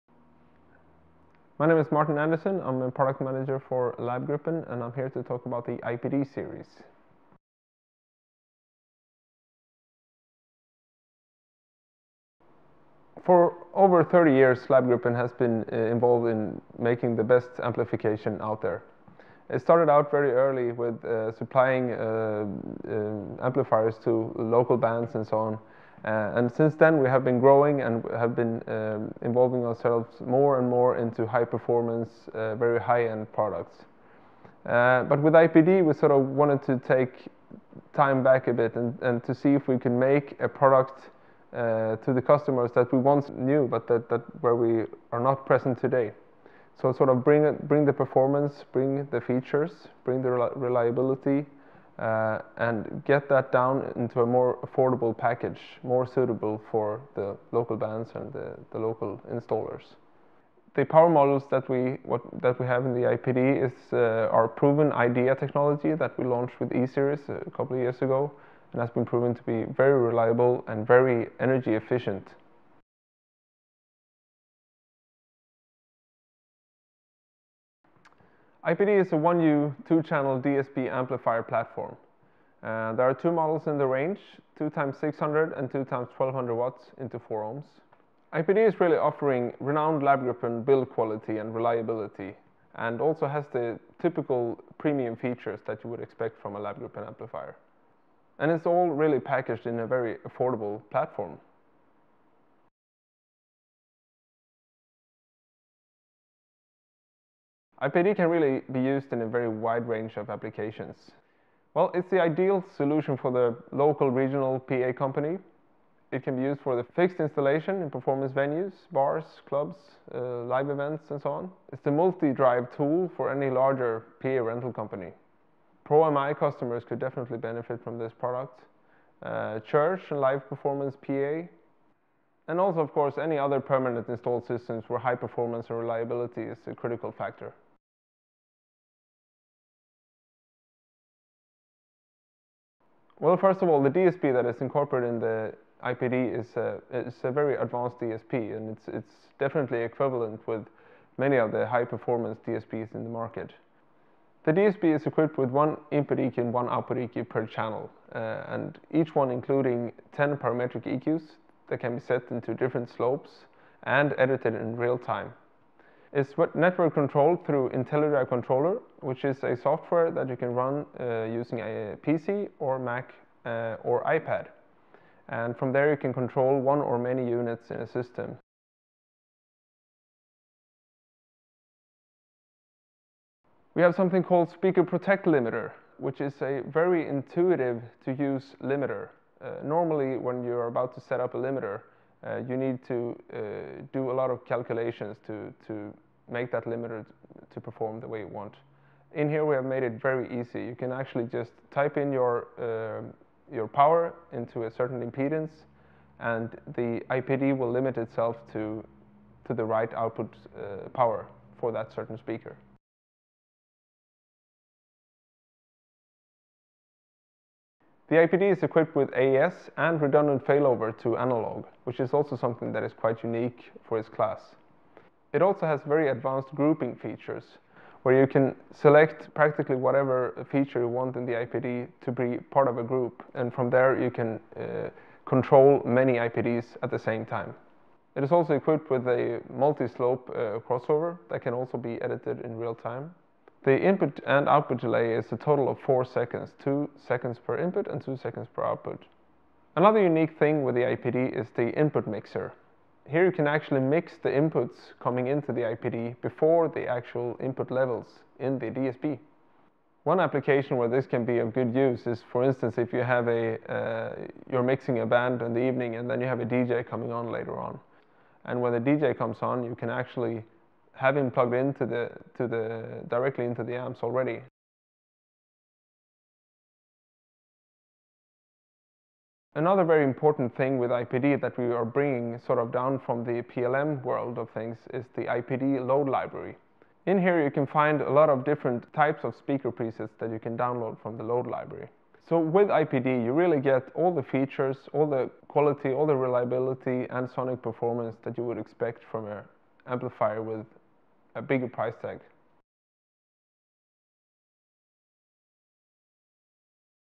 IPD Series - Finally, 1U really can have it all.
Lab.gruppen's revolutionary IPD Series intelligent amplifiers harness three decades of advanced amplifier design and experience in high-end performance touring amplication, distilling it all into a brand new compact 1U, feature-rich amplifier platform that's equally at home in small-medium scale live PA as it is in performance install applications. Here, Product Manager for Lab.gruppen, Martin Andersson, introduces the concept behind the product.